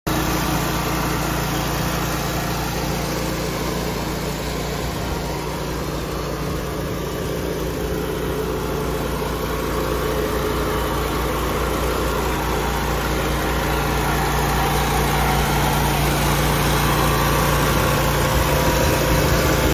2016 EXMARK LZX801GKA606B1 For Sale
2016 Exmark LazerZ on our lot. This zero turn features a 60" deck, Kawasaki motor and suspension seat. This mower operates as it should. Contact our sales team with any questions.